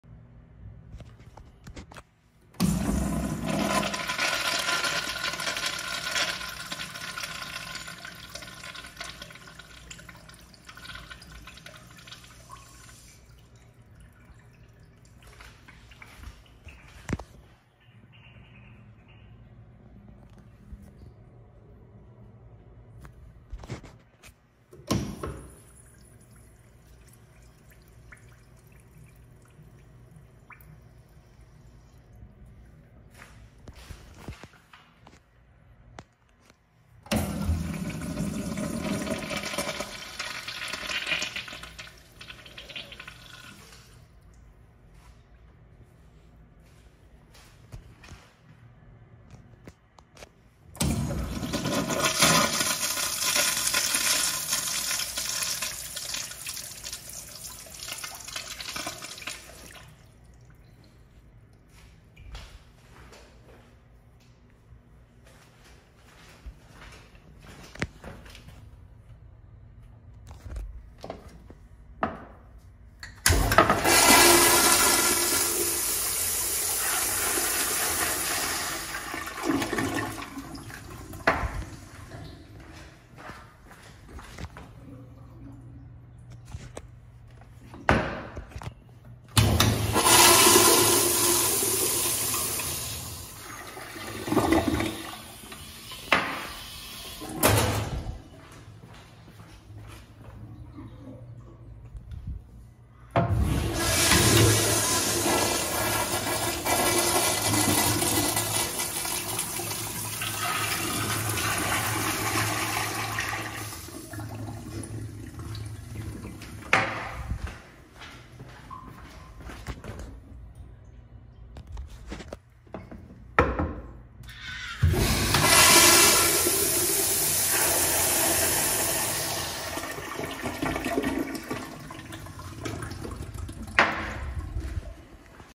Marriott Resort Sanibel Harbour Restroom Full Shoot
75% of these fixtures are original, with the other 2 being replacements from the early-late-2000s